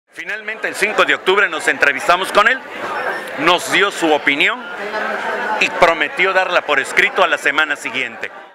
Jubilados solicitan al gobernador tocar tema de adeudos al IPE
El secretario general suplente de la Delegación de Jubilados del SNTE, Rodolfo Suárez Castellanos, solicitó ante los medios de comunicación al gobernador Javier Duarte de Ochoa dar prioridad al pago de los adeudos que la administración mantiene con el Instituto de Pensiones del Estado (IPE).